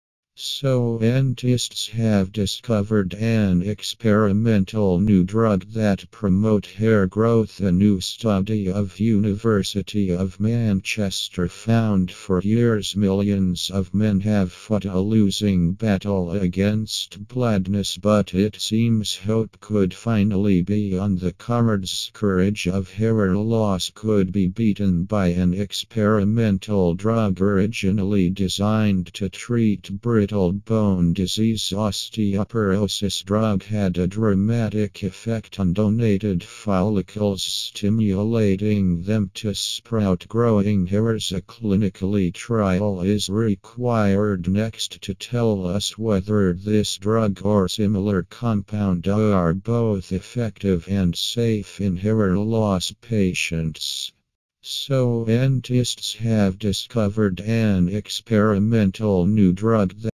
SCIENTISTS HAVE DISCOVERED A NEW DRUG PROMOTE HAIR GROWTH
A STUDY OF UNIVERSITY OF MANCHESTER FOUND SCOURAGE OF HAIR LOSS COULD BE BEATEN BY AN EXPERIMENTAL DRUG ORIGINALLY DESIGNED TO TREAT BRITTLE BONE DISEASE OSTEOPOROSIS DRUG A DRAMATIC EFFECT ON DONATED FOLLICLES STIMULATING SPROUT GROWING HAIRS